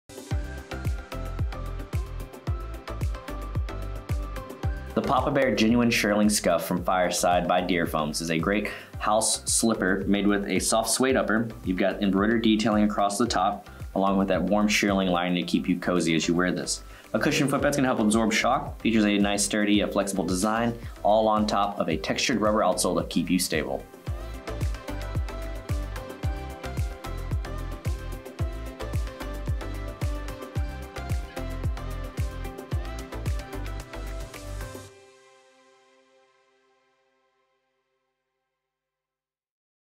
FIRESIDE by Dearfoams Papa Bear Genuine Shearling Scuff SKU: 9779285
Keep your feet warmed up in the soft and cozy FIRESIDE by Dearfoams® Papa Bear Genuine Shearling Scuff slippers.
Leather upper.
Leather lining and cushioned insole.
Slip-on style.
Papa Bear script on the upper.
Real shearling from lamb in an artificially dyed treatment originated from Australia.
Synthetic outsole.
Imported.